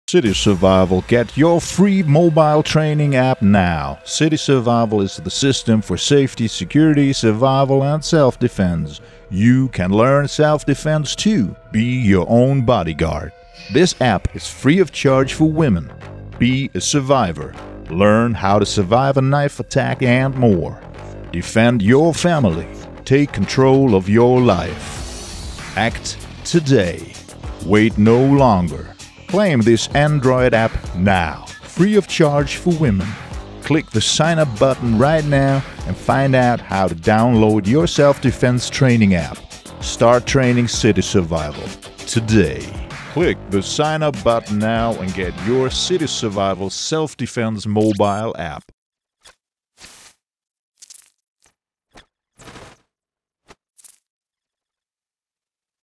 City Survival Mobile App - Self_defense app (Android) Be your own Bodyguard
Learn how to defend yourself by use of our mobile app for Android.
City Survival has been developed by prof close protection operator in over 30 years - field tested by experts. YOU can use these combatives too, the best of Krav Maga, KAPAP and other sysstems, condensed into your system for security, safety, survival and self-defense!